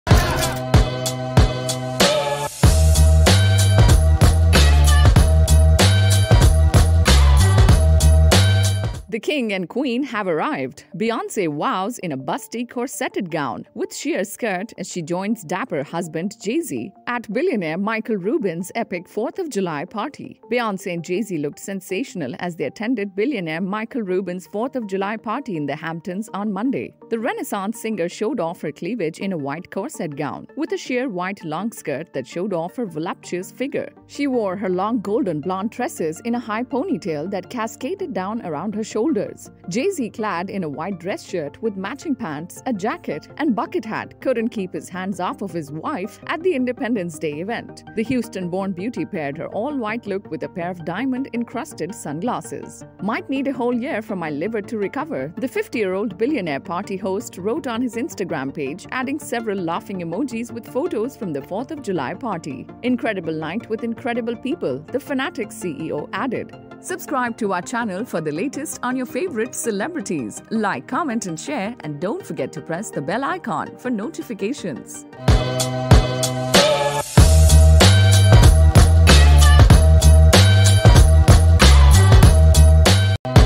Beyonce and Jay Z attend Michael Rubin's 4th July party in a busty corseted gown
Beyoncé, and Jay-Z looked sensational as they attended billionaire Michael Rubin's 4th Of July party in the Hamptons on Monday.